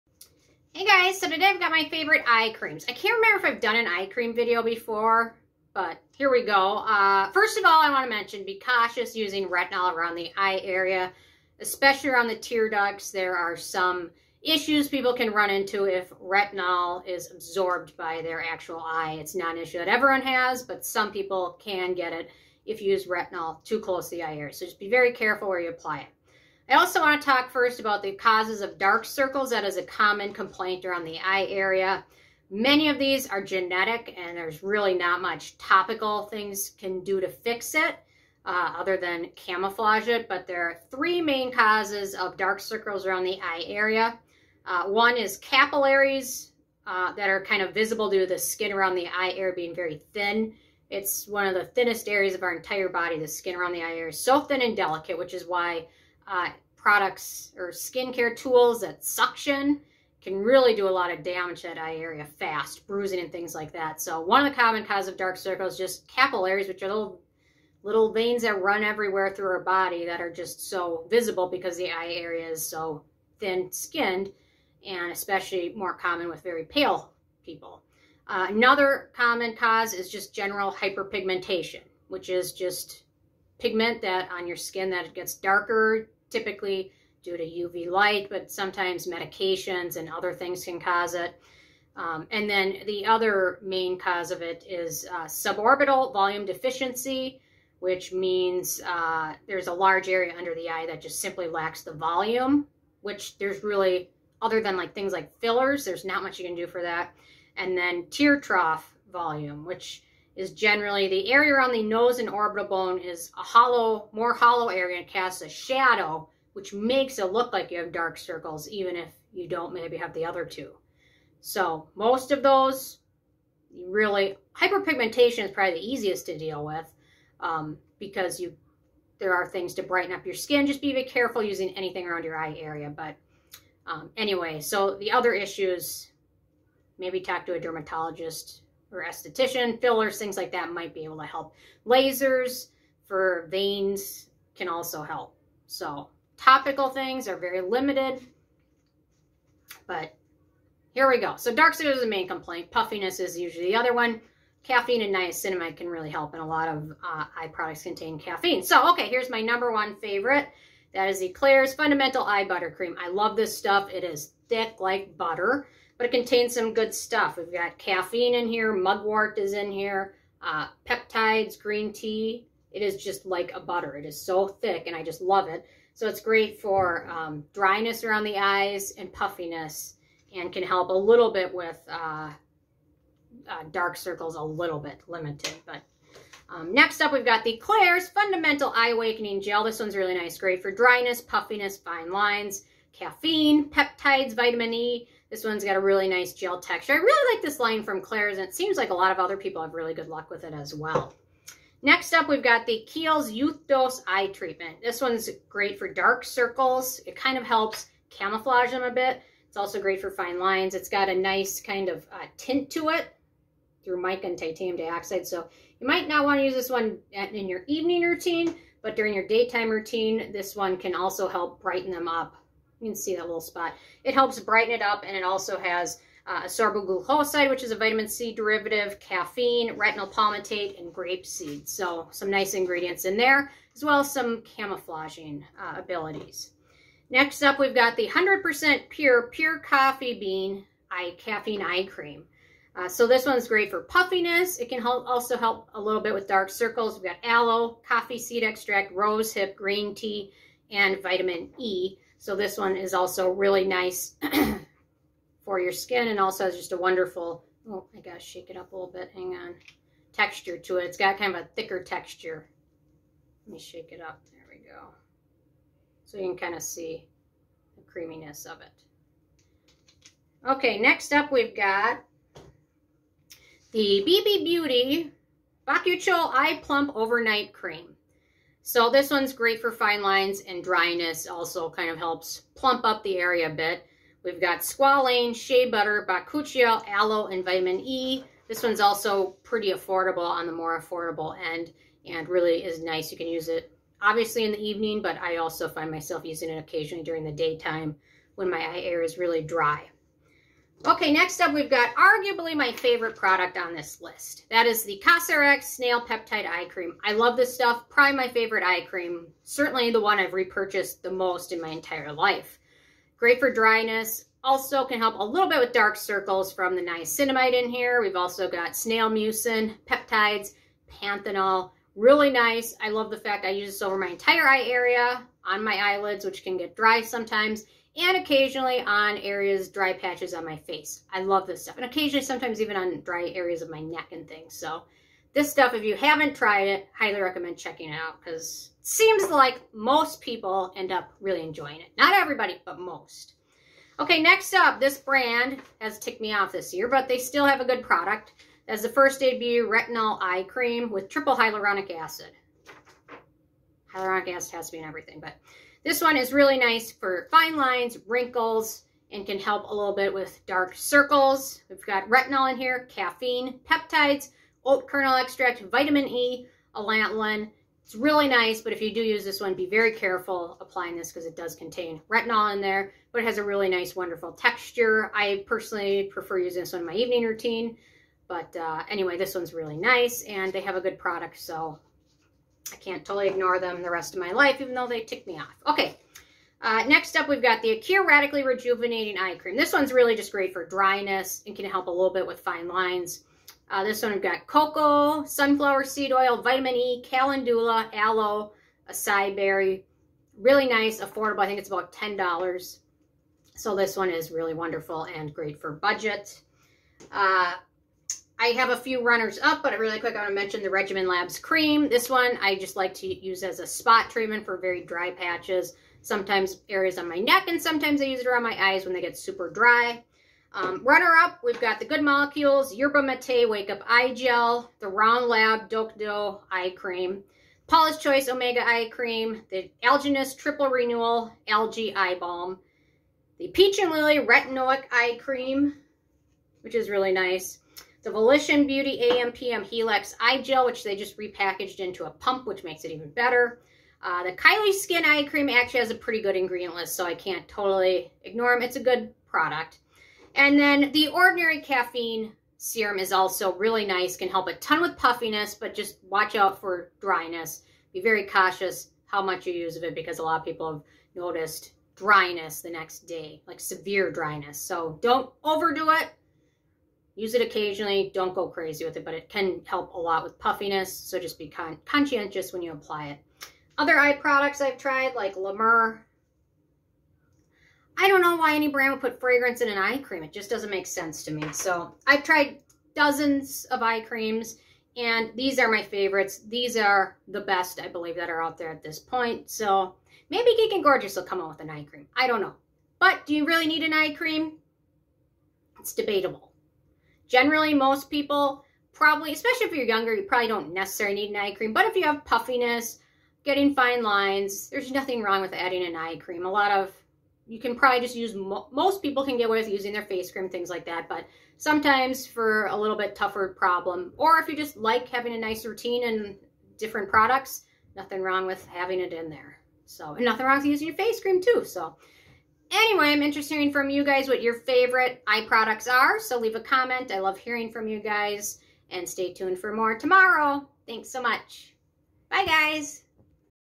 Best Eye Creams & Serums 👀 Dark Circles- Puffiness- Dryness- Crows Feet 😍 Fragrance Free!
Favorite Eye Creams:

***Be very cautious using Retinol around the eye area and stay away from tear ducts!***
If you are interested in reading more about Retinol and potential eye issues (such as permanent dry eye disease,

Causes of Dark Circles: Many of these causes can be genetic, which means effective topical solutions are limited outside of camouflaging them.
- Hyperpigmentation- darkening of skin around the eye area.
- Tear Trough Shadow/Suborbital Volume Deficiency- tear trough shadow:  area around the nose and orbital bone is hollowed that casts a shadow giving the look of dark circles.  Suborbital volume deficiency means there is a large area under the eye that simply lacks volume.
- Capillaries due to the skin around the eye area being very thin.  Not much topical products can do for this, occasionally a laser can be used to reduce the visability of the caplillaries.

My Favorite Eye Creams List:
Klairs Fundamental Eye Butter Cream
Great for puffiness, dryness, fine lines
Caffeine, Mugwort, Green Tea, Peptides
Use Rewards Code NOBSBEAUTY for an additional discount!

Klairs Fundamental Eye Awakening Gel
Great for dryness, puffiness, fine lines
Caffeine, Peptides, Vitamin E

Kiehl's Youth Dose Eye Treatment
Great for dark circles, fine lines
Ascorbyl Glucoside, Caffeine, Retinyl Palmitate, Grapeseed

100% Pure Coffee Bean Caffeine Eye Cream
Great for puffiness, dark circles
Aloe, Coffee Seed Extract, Rosehip, Green Tea, Vitamin E
At Amazon (fulfilled by 100% Pure)

Bybi Beauty Bakuchiol Eye Plump Overnight Cream
Great for fine lines, dryness
Squalane, Shea Butter, Bakuchiol, Aloe, Vitamin E
At Target

Cosrx Snail Peptide Eye Cream
Great for dryness, dark circles
Snail mucin, Niacinamide, Peptides, Panthenol

First Aid Beauty Retinol Eye Cream
Great for fine lines, wrinkles, dark circles
Retinol, Caffeine, Peptides, Oat Extract, Vitamin E, Allantoin

Acure Radically Rejuvenating Eye Cream
Great for dryness, fine lines
Cocoa, Sunflower Seed Oil, Vitamin E, Calendula, Aloe, Acai
At Target

Runner Ups:
Good Molecules Yerba Mate Wake Up Eye Gel
Round Lab Dokdo Eye Cream
Paula’s Choice Omega Eye Cream
Algenist Triple Algae Eye Renewal Balm
Peach & Lily Retinoic Eye Cream
Volition Beauty AM PM Helix Eye Gel
Kylie Skin Eye Cream
The Ordinary Caffeine Serum